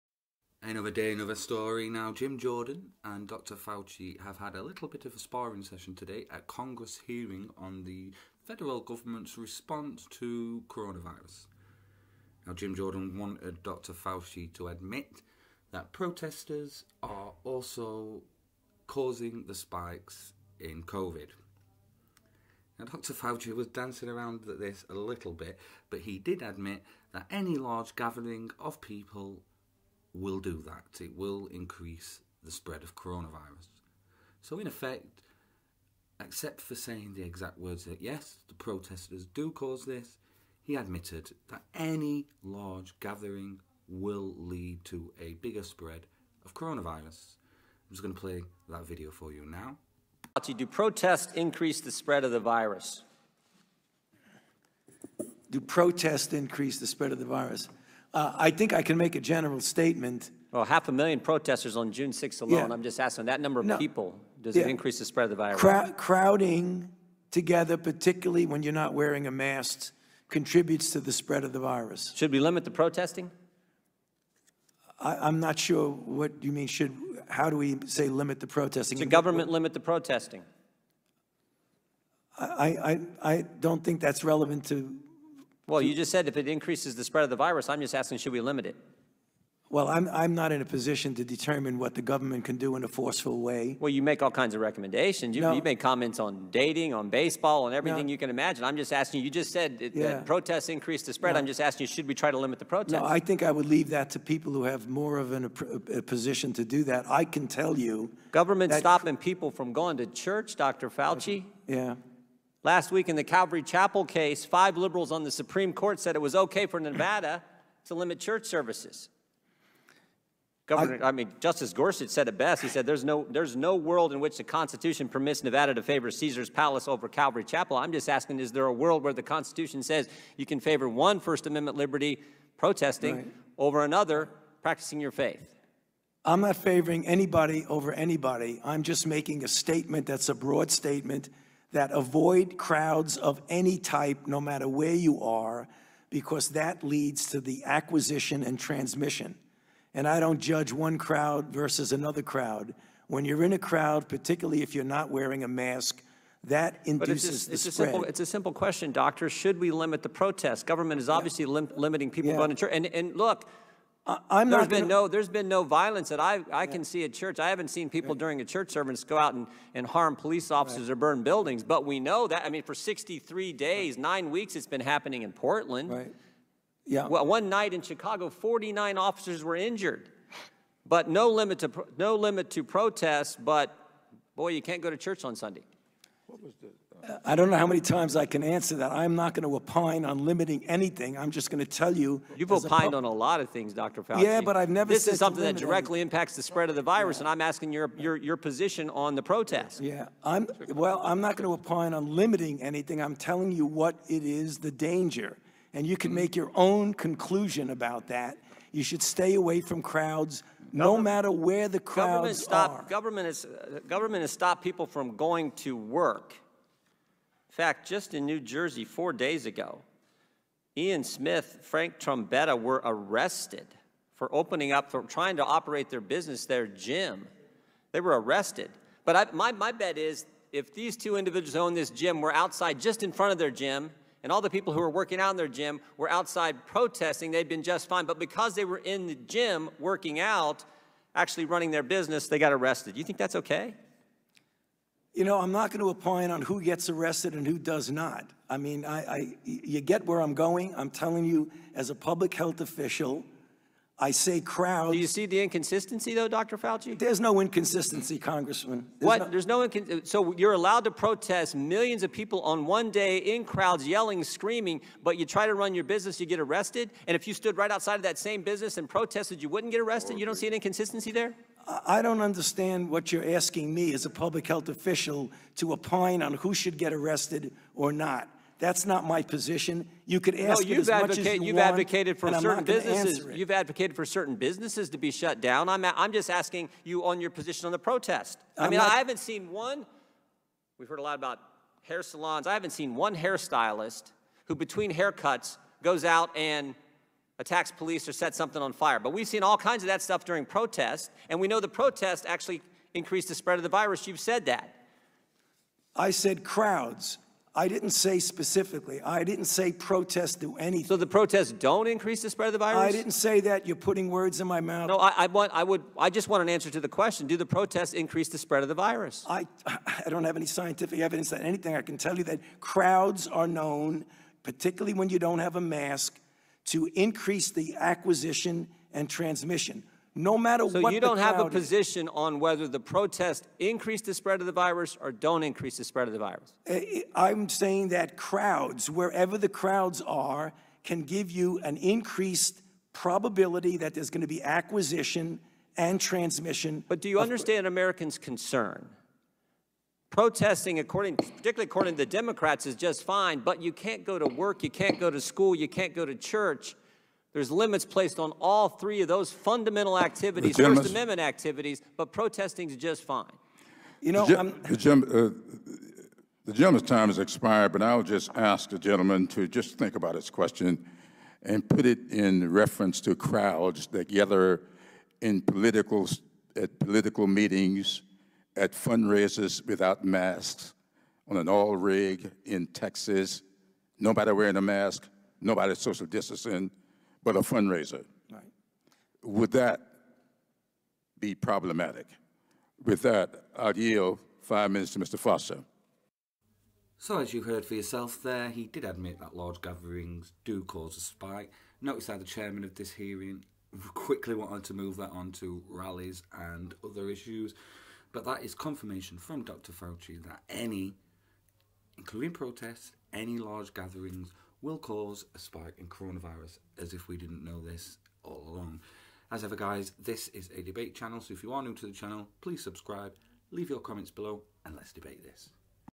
DR FAUCI ADMITS PROTESTS DO CAUSE SPIKE IN COVID 19 SORT OF
at a hearing on the coronavirus response today jim jordan and dr fauci had a brief discussion on protests leading to spike in covid 19